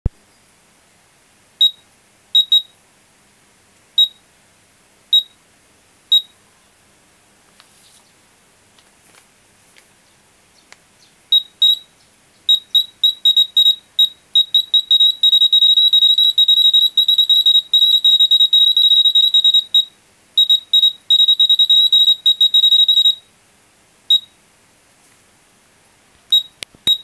パンケーキGM管の鳴り具合
アルファ、ベータ線検出を目的として、パンケーキ型のGM管を買ってみました。
先日買った謎の線量計、相変わらず具合の悪いのが１台あって、ちょうどバラック状態だったのでこの謎の線量計につないでみました。

反応すごくいいです。

これでなんとか食品の表面汚染測れないかなぁ・・・